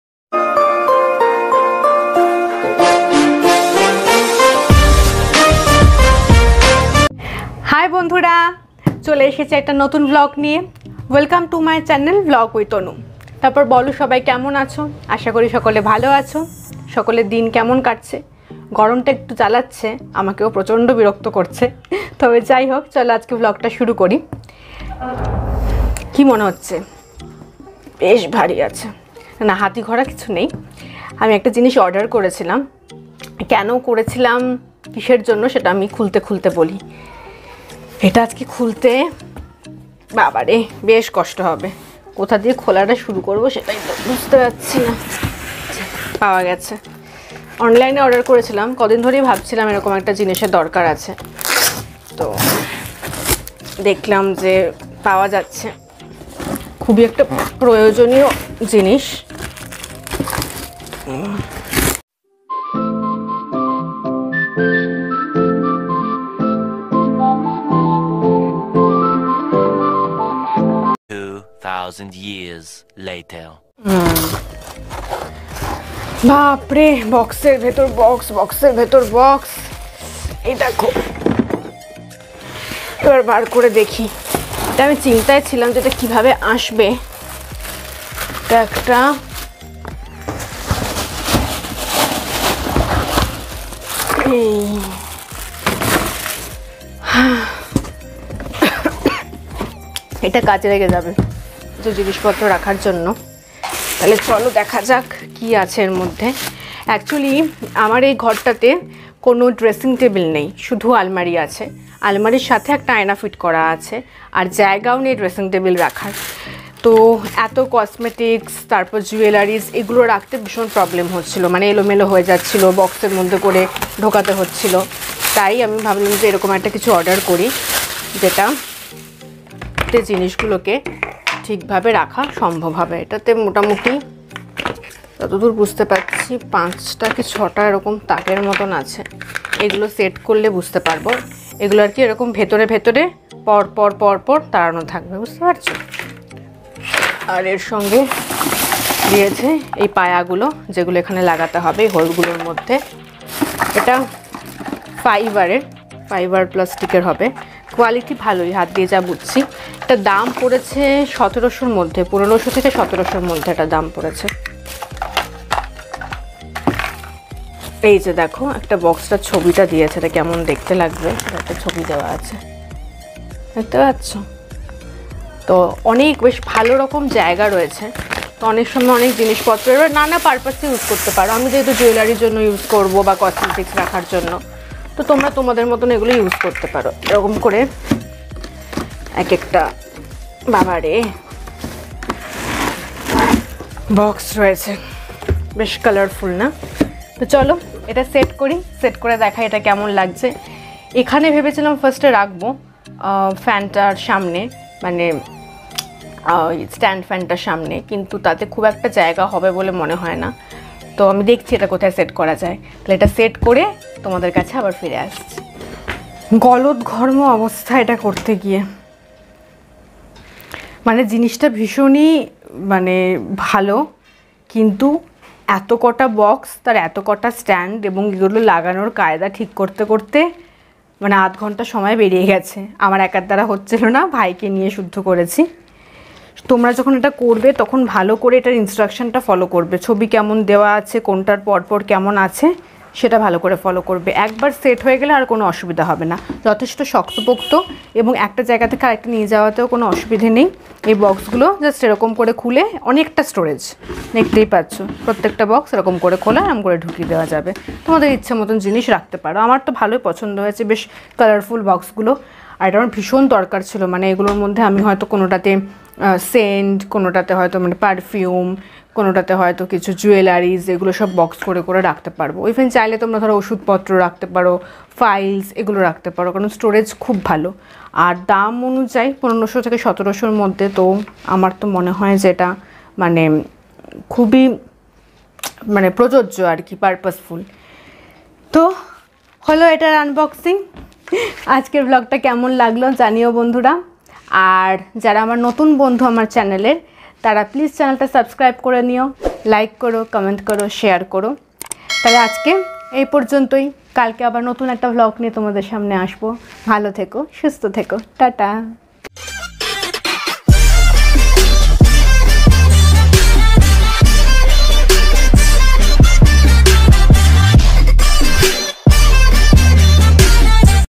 What's Inside This Box From Flipkart ?? | Big Box Mystery | Vlog With Anu
Take a look at this Mahadev Enterprise Plastic Free Standing Chest of 5 Drawers/Extra Large Multi-Purpose Modular Drawer Storage Organizer for Home, Office, Hospital, Parlour and School Foldable Storage Box Cutlery Kitchen Rack on Flipkart